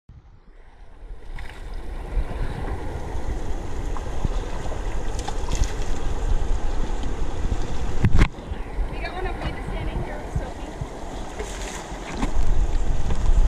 River bathing with elephants with Sophie & Lucky
@ The Siam River Rafting homestay